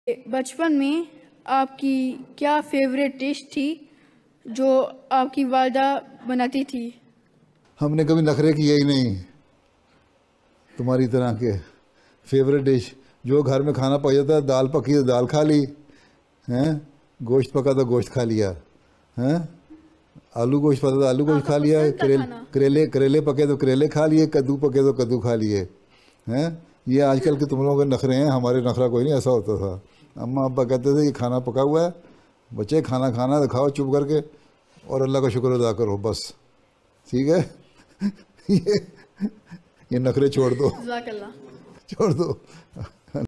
Beloved Huzoor, what was your favorite dish that your mother would cook for you?
On 28 August 2021 Beloved Huzoor Hazrat Mirza Masroor Ahmad (May Allah be his Helper) took a class with Atfal of Germany.